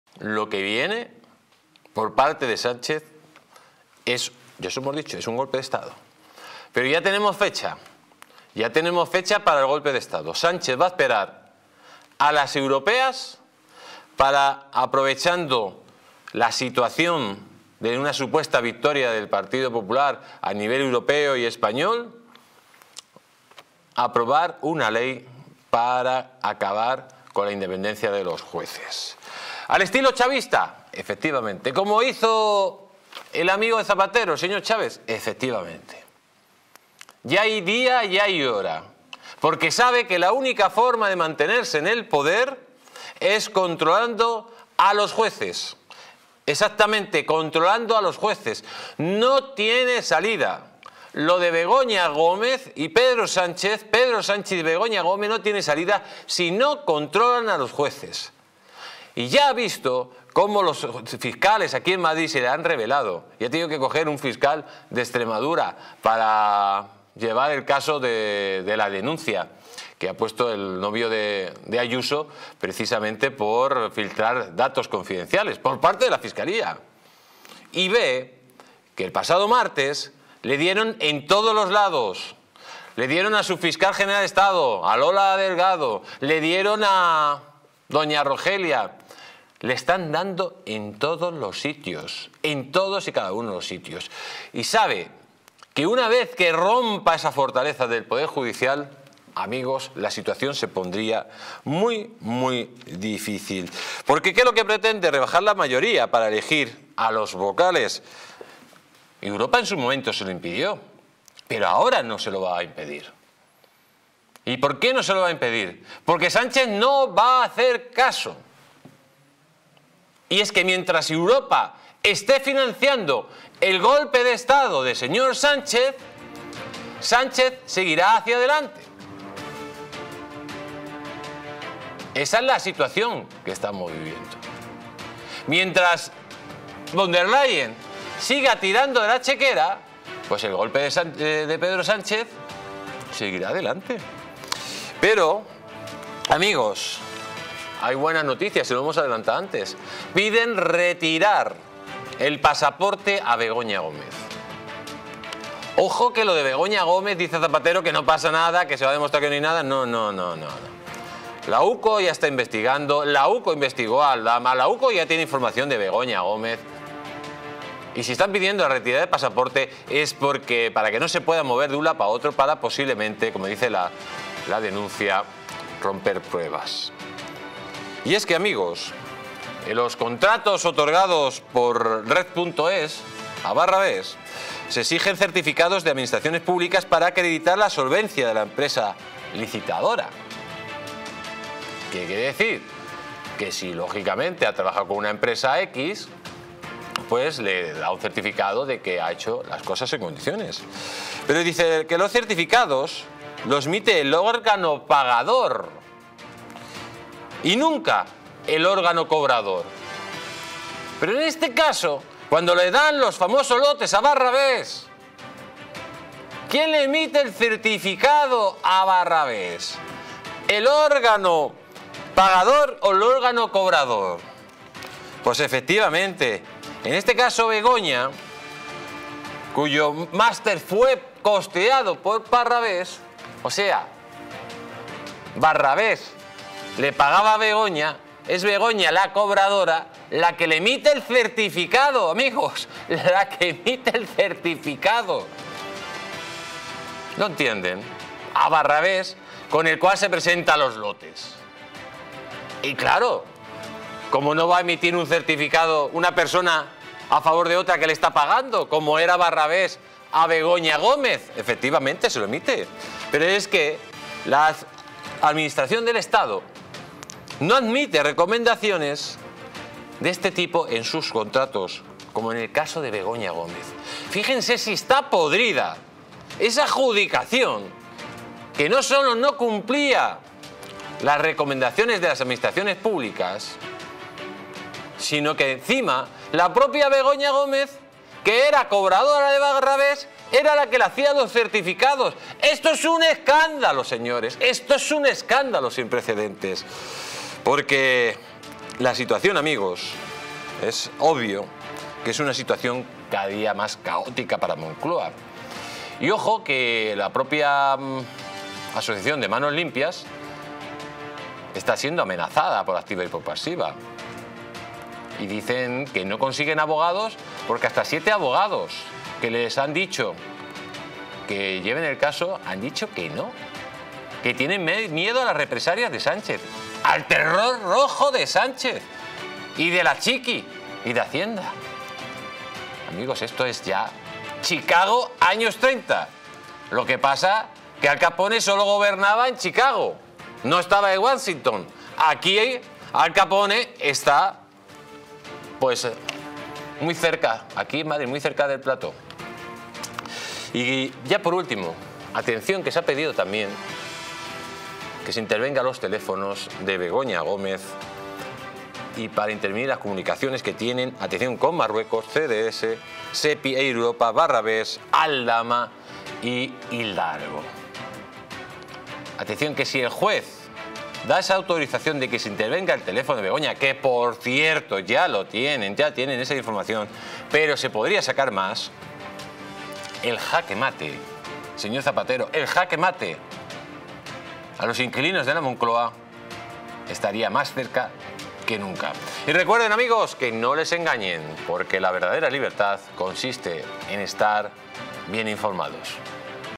🔴EDITORIAL de DÍA🔴INFARTO EN LA MONCLOA, DESCUBREN UN DOCUMENTO CLAVE QUE ES MORTAL PARA BEGOÑA🔴
En esta editorial de día, descubrimos un documento clave que puede ser mortal para Begoña. No te pierdas esta noticia impactante en Distrito TV. ¡Mantente informado con nosotros! 🔴📰distritotv
🔴Conviértete en miembro de este canal para disfrutar de ventajas: